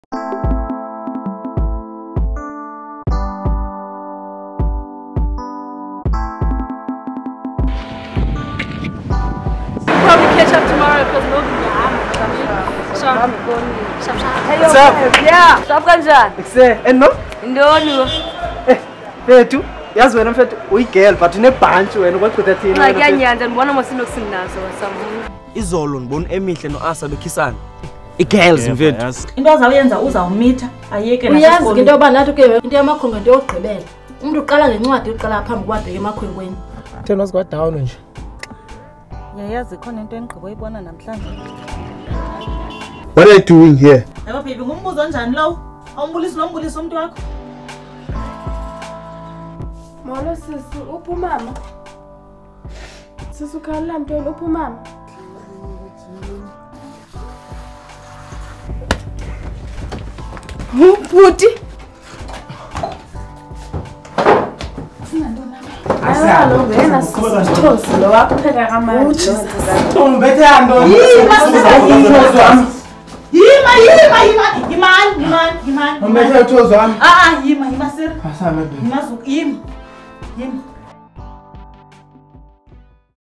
Transkei/Eastern Cape Films (December 2014 release)
Short films produced by King Zukha team. Screening eDutywa, Butterworth and surrounding areas. Please show some love.